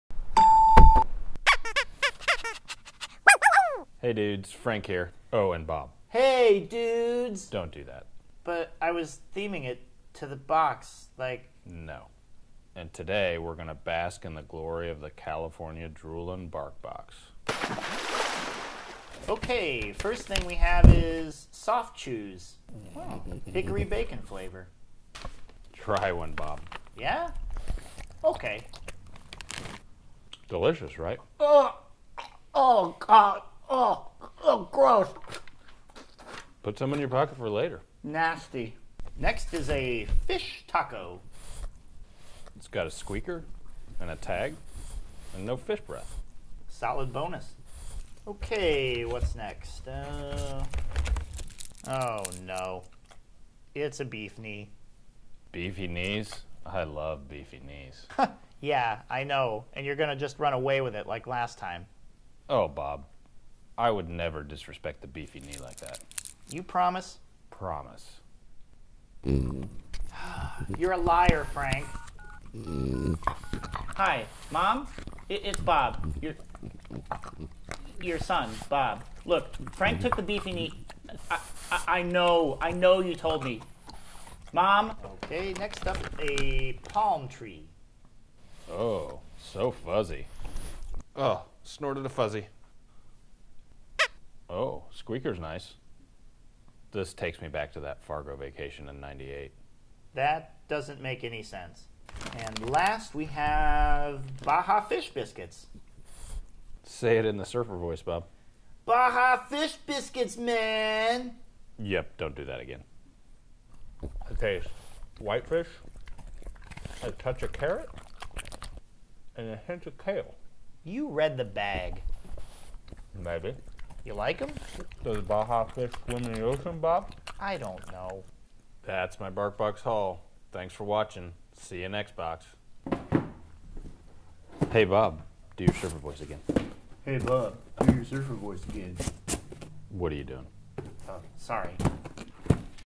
California Droolin' Bark Box Haul with Frank
Hear it straight from the dog's mouth as Frank unboxes his monthly Bark Box goodies!